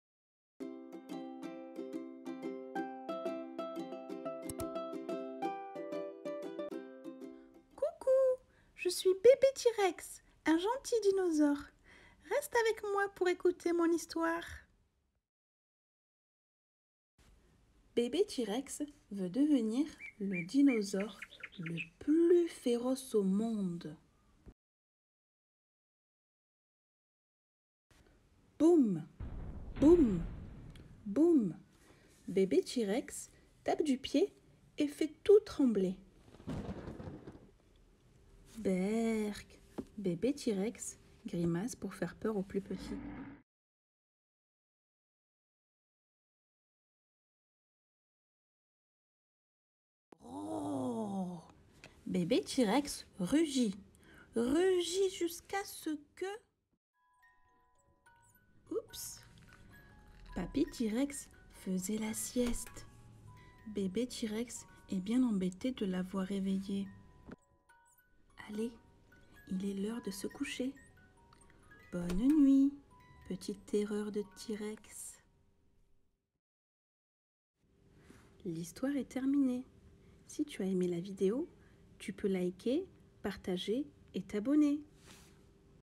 Bébé T-Rex , livre marionnette : histoire du soir racontée ￼🧚‍♂️🎠
Bébé T-Rex aime faire peur. C'est une vraie terreur ! Mais lorsqu'il rugit, il réveille son papi ! Ce n'est qu'une fois endormi qu'il n'y a plus de bruit. Être terrifiant, c'est épuisant !Un petit récit à raconter et à jouer par les parents, une histoire qui prend vie grâce à une chouette marionnette pour une lecture pétillante et rigolote.Le tout-petit devient l'acteur enthousiaste d'une première lecture très interactive dans un livre qu'il peut s'approprier.

Un livre de Victoria Ying
editions : Casterman

Mes autres vidéos :

Les Trois Petits Cochons 🐷🐺